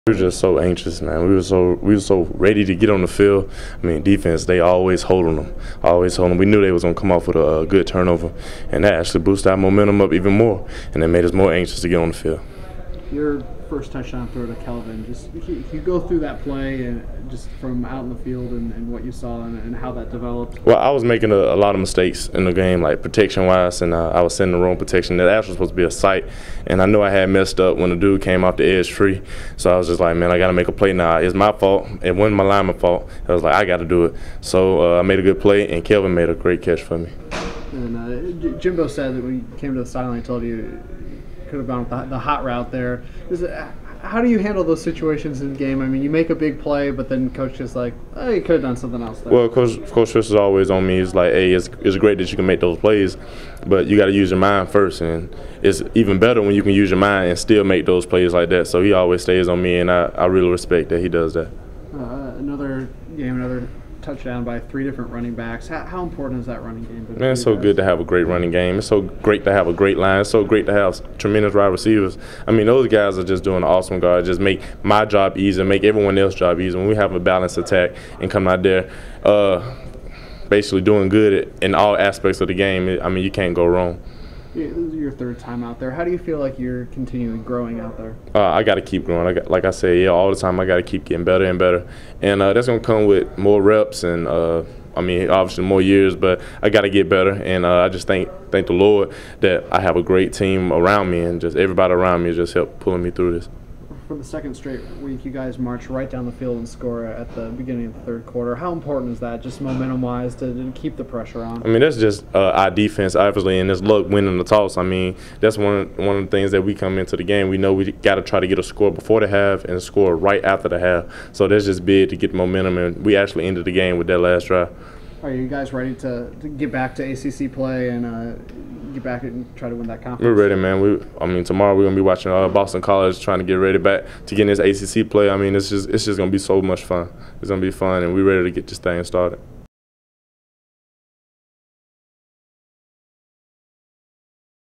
Jameis Winston Postgame: BCU
Courtesy Florida State quarterback Jameis Winston talks with Seminoles.com following Florida State's win against Bethune-Cookman.